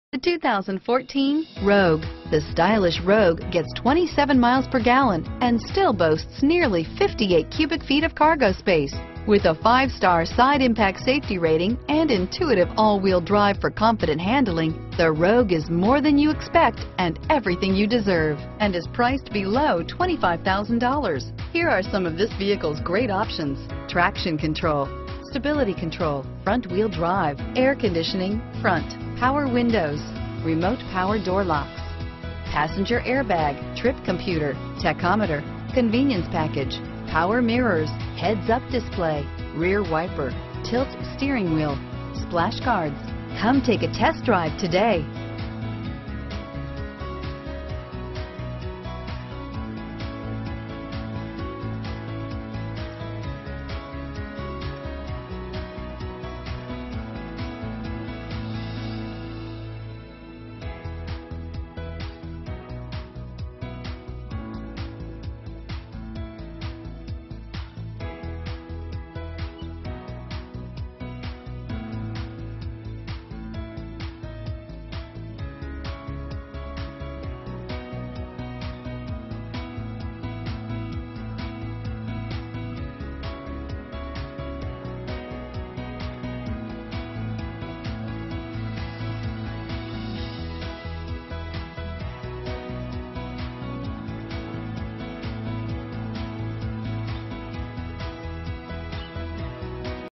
2014 Nissan Rogue Select Morton Grove, Des Plaines, Glenview, Evanston ,Mt Prospect, IL 20
2014 Nissan Rogue Select S - Stock#: 20530 - VIN#: JN8AS5MT9EW612738

Star Nissan
5757 W Touhy Ave

2014NissanRogue SelectS2.5L 4 cylsCVT (Continuously Variable)Brilliant SilverBlack w/Cloth Seat Trim Optional equipment includes: Convenience Package Floor Mats & Cargo Area Protector Splash Guards...Standard features include: Remote power door locks Power windows with 1 one-touch CVT Transmission 4-wheel ABS brakes Air conditioning Traction control - ABS and driveline Power mirrors Tilt steering wheel 170 hp horsepower 2.5 liter inline 4 cylinder DOHC engine Head airbags - Curtain 1st and 2nd row Passenger Airbag 4 Doors Front-wheel drive Fuel economy EPA highway (mpg): 28 and EPA city (mpg): 23 Tachometer External temperature display Interior air filtration Trip computer Clock - In-dash Front seat type - Bucket Rear bench seats Rear wiper Stability control...Great safety equipment to protect you on the road: ABS Traction control Curtain airbags Passenger Airbag Stability control...It has tons of features such as: Power locks Power windows CVT Transmission Air conditioning Power mirrors...